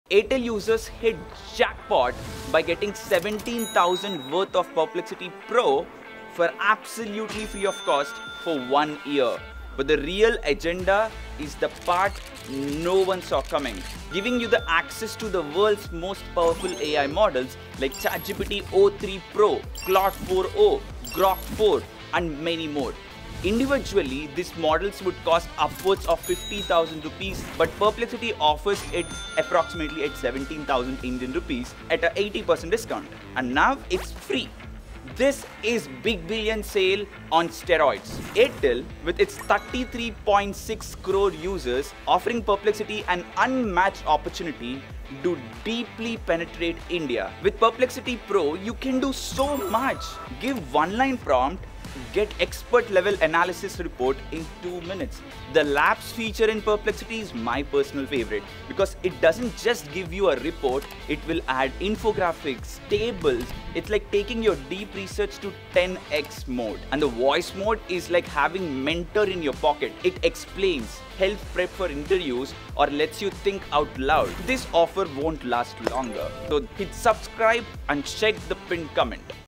Airtel Users Get Perplexity Pro FREE (₹17K AI Power) 🚀🔥
Follow the below steps to get Perplexity Pro:

Step 1 – Open Airtel Thanks App and log in.

Step 2 – Go to the Rewards section and select Perplexity Pro.

Step 3 – Click on Claim Now and proceed.

Step 5 – Enter the OTP to claim the code for Perplexity Pro.

Step 7 – Use the OTP and code to activate Perplexity Pro for free for 12 months.

🎁 BONUS 🎁

Aravind Srinivas (CEO of Perplexity) just dropped pure gold on the future of AI and what’s coming will blow your mind 🤯

Happy learning✌️